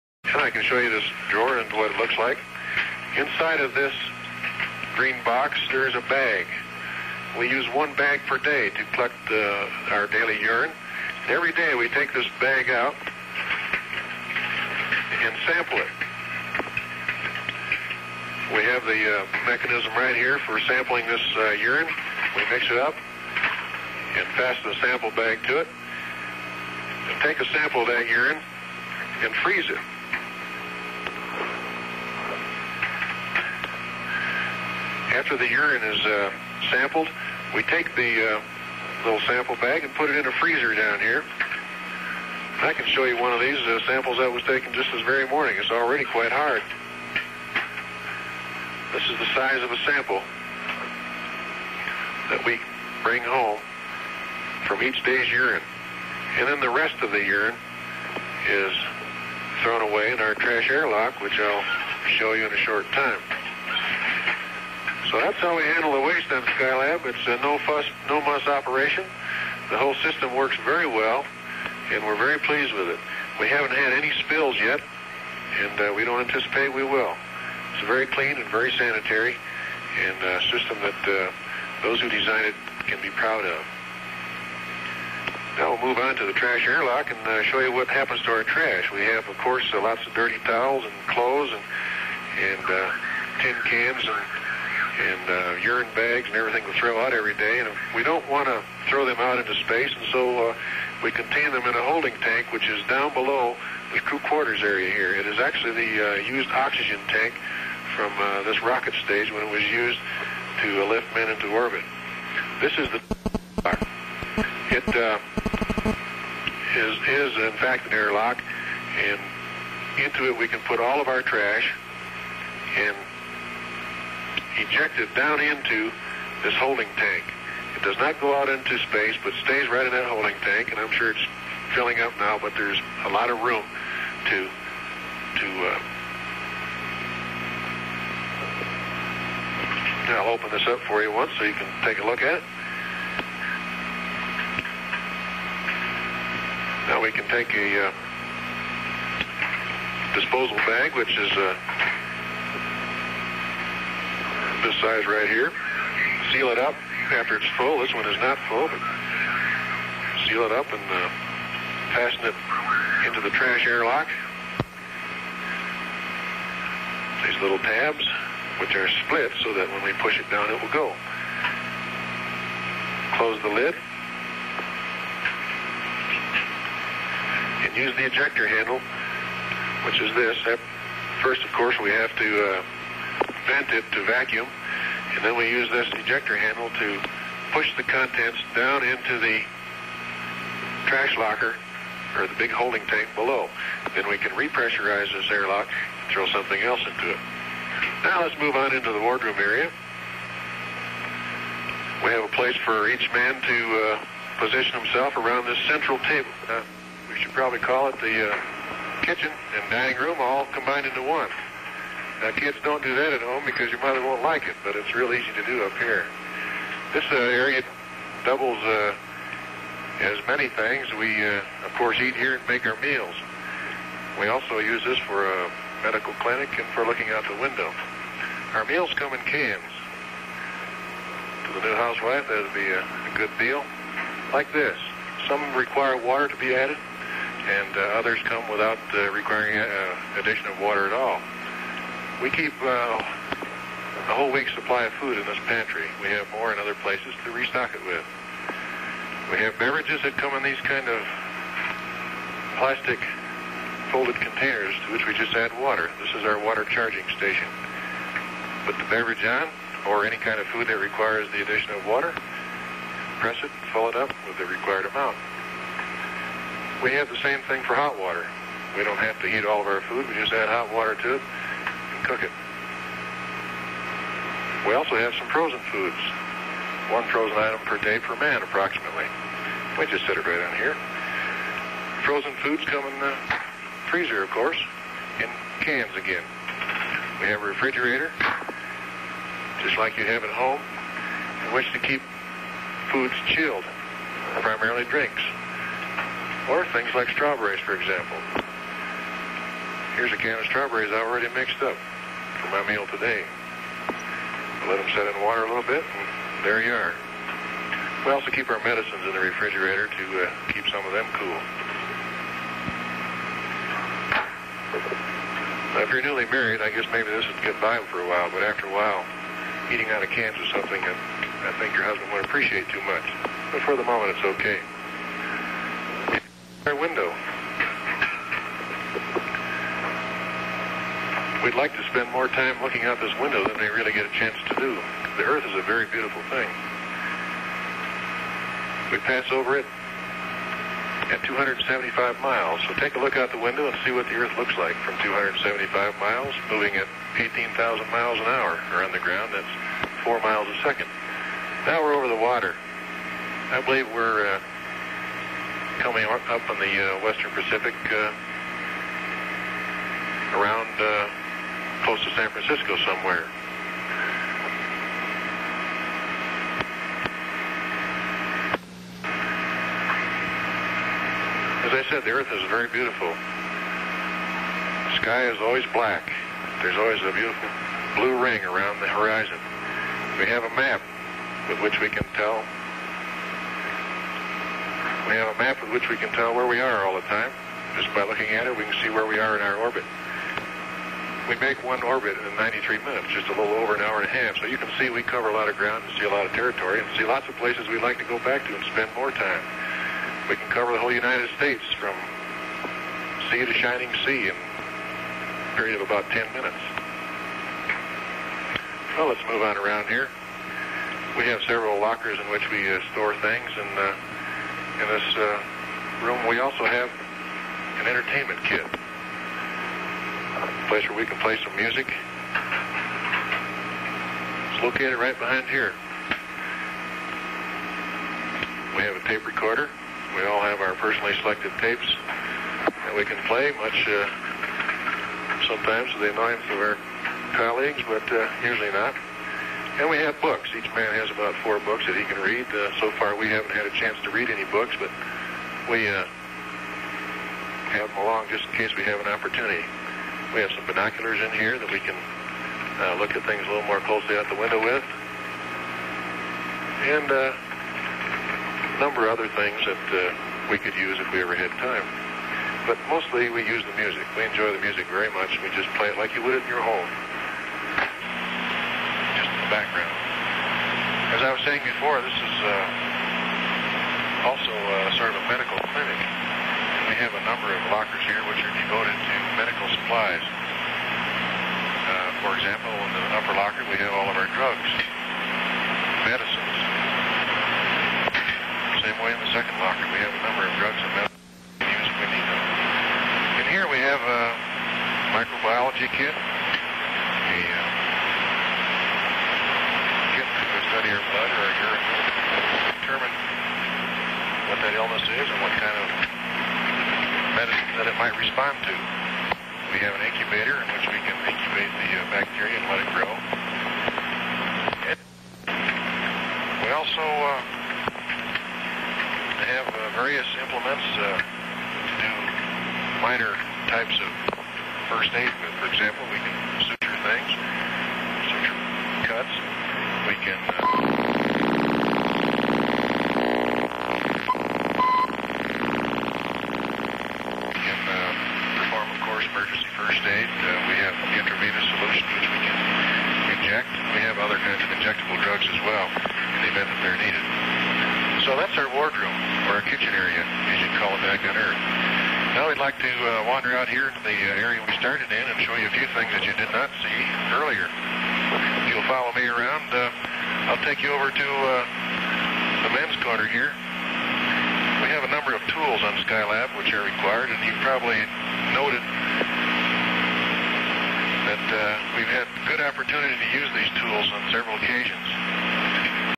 Skylab 3 - Tour of Skylab (PT 4)
Astronaut Jack Lousma takes viewers on a tour of the Skylab Space Station in this 45 minute broadcast. Part 4 of 5.